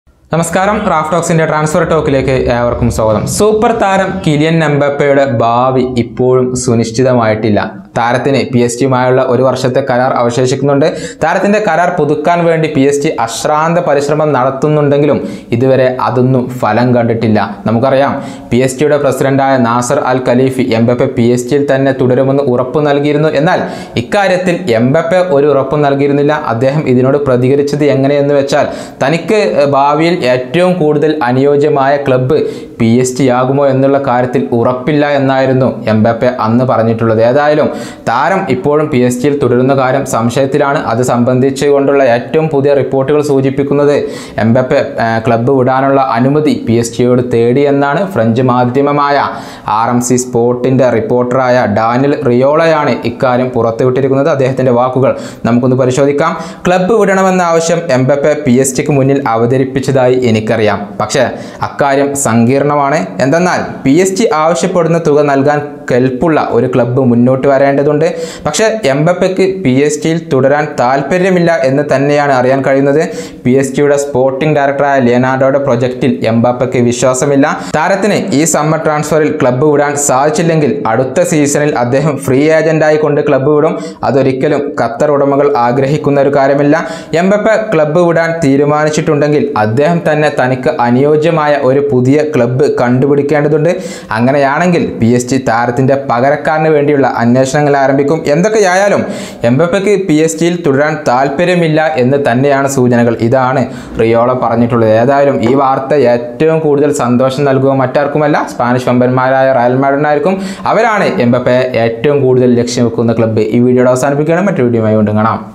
പിഎസ്ജി വിടണമെന്ന് എംബപ്പേ ആവിശ്യപ്പെട്ടു, റിപ്പോർട്ട്‌! | Transfer News
ട്രാൻസ്ഫർ റൂമർ
Kylian Mbappe